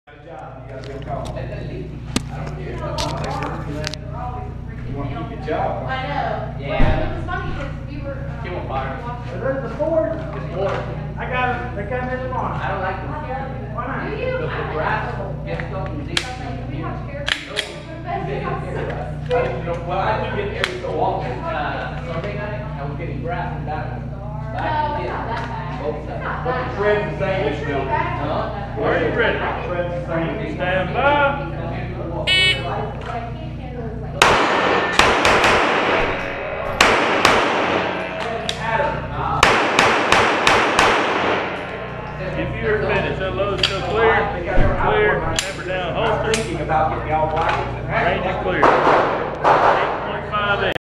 GPSL Stage 2 - Time to Burn? - July 19, 2016
Stage 2 at the July 19, 2016 Gwinnett Practical Shooting League match, which is held at Range, Guns & Safes in Norcross.

An interesting stage with a SLOWLY appearing target. Waiting for it as long as I did definitely hurt a bit.

Glock 34
Dawson sights